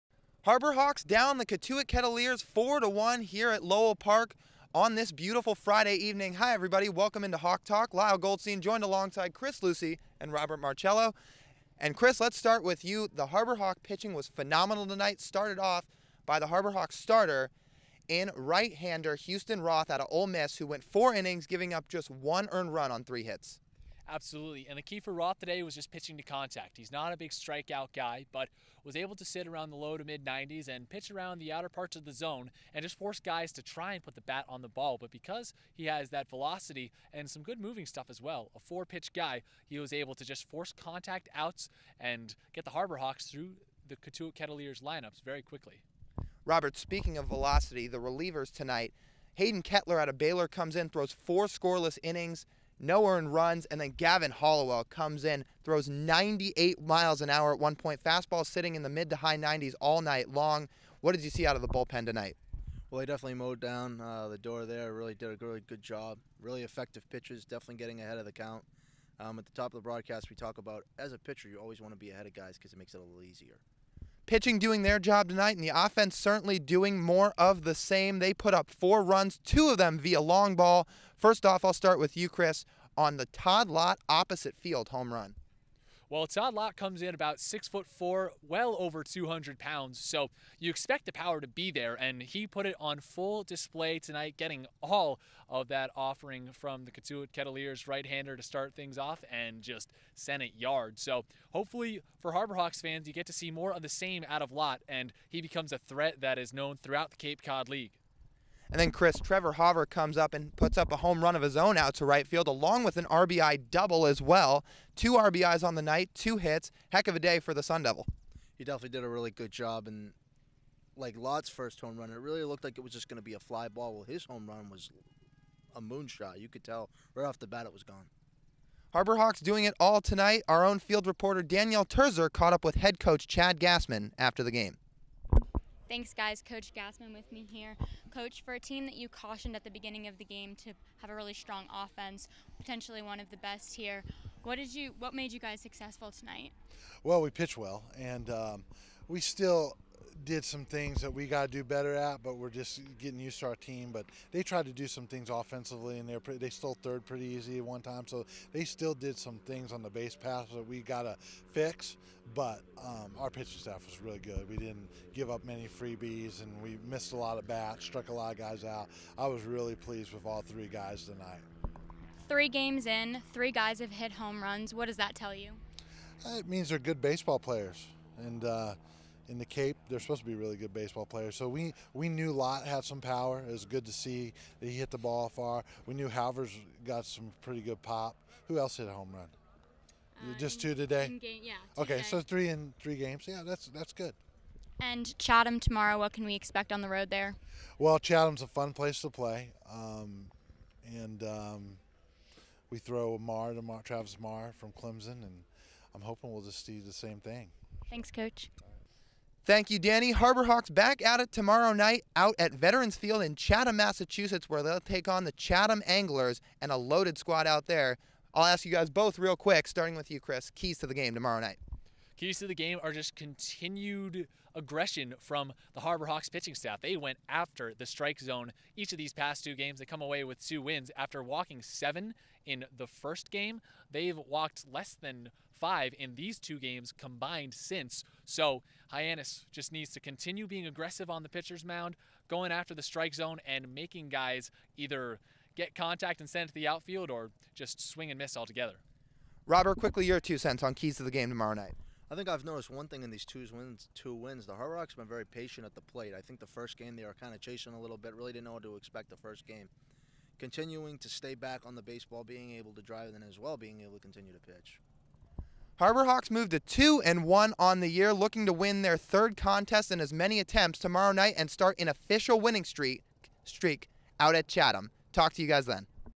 Hawk Talk: Hyannis vs. Cotuit, Fri. 6/15/18
Lyle Goldstein, Chris Lucey and Robert Marcello break down a 4-1 Harbor Hawks win in game 1 of the Barnstable Patriot Cup Series. Reporter Dani Terzer catches up with Hyannis' manager Chad Gassman.